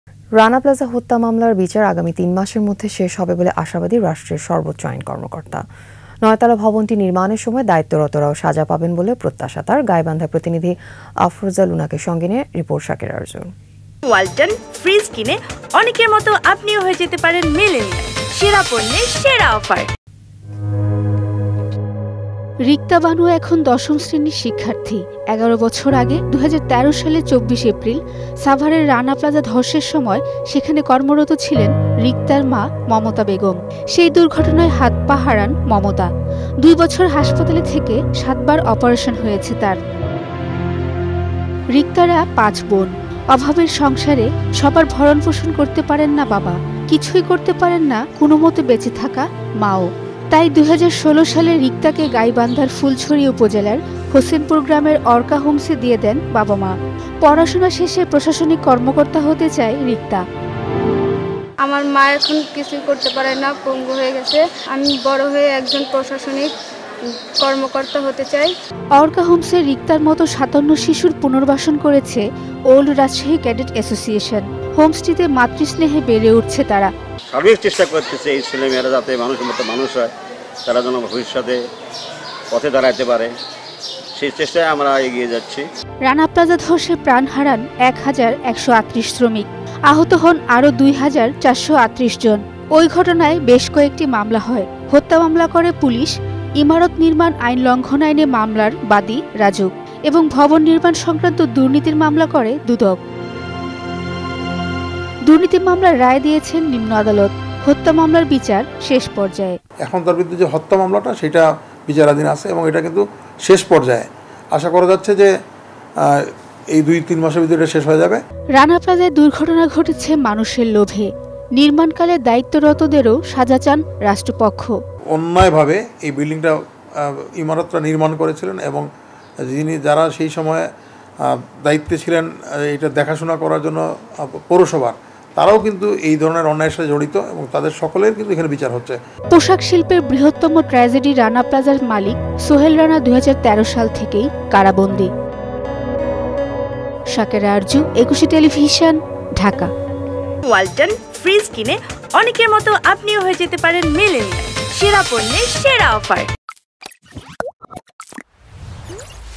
রানা প্লাজা হত্যা মামলার বিচার শেষ হবে আগামী ৩মাসে | ETV News
রানা প্লাজা হত্যা মামলার বিচার আগামী তিনমাসের মধ্যে শেষ হবার আশাবাদী রাষ্ট্রের সর্বোচ্চ আইন কর্মকর্তা। নয়তলার ভবনটি নির্মাণের সময় দায়িত্বরতরাও সাজা পাবেন বলেও প্রত্যাশা তার। গাইবান্ধা প্রতিনিধি আফরোজা লুনা কে সঙ্গে নিয়ে শাকেরা আরজুর রিপোর্ট।